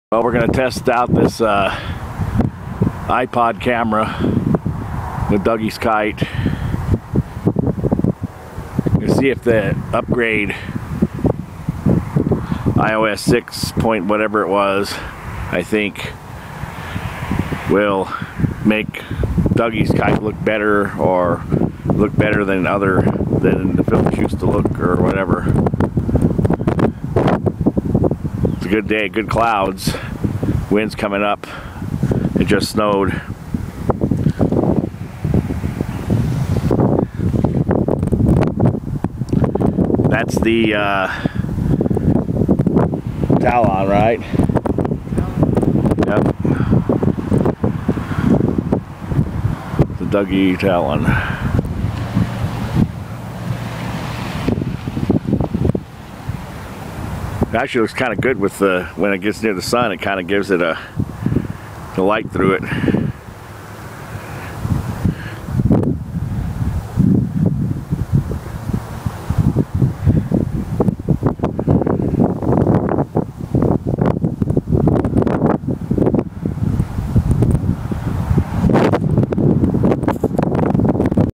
Talon stunt kite
Doug field flying at Great Lawn Park